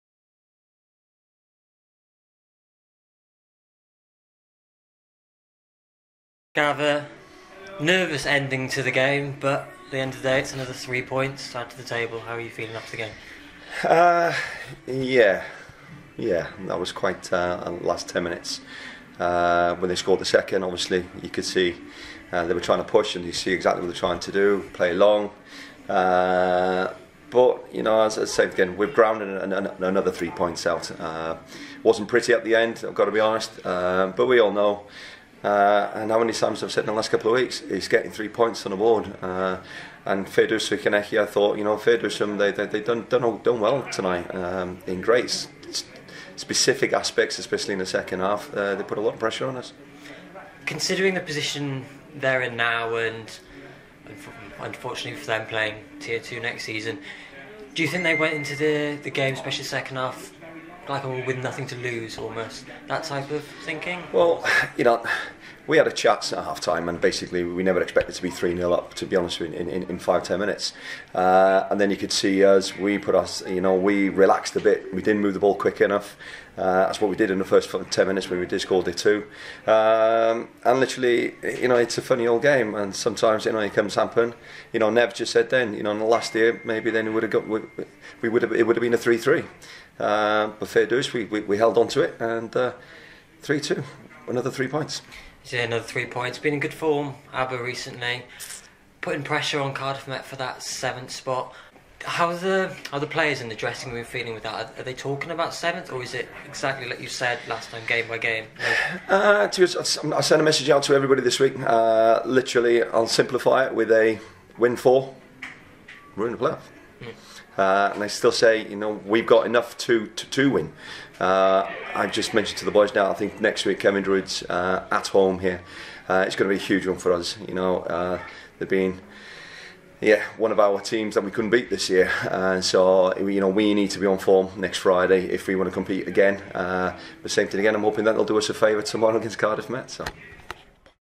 INTERVIEW | Gavin Allen after Aber's 3-2 win against Llanelli
Gavin Allen after Aber Town's 3-2 home win against Llanelli on 5th April 2019.

Interviewed, Filmed and Edited by Steven Brookes
Photos by Colin Ewart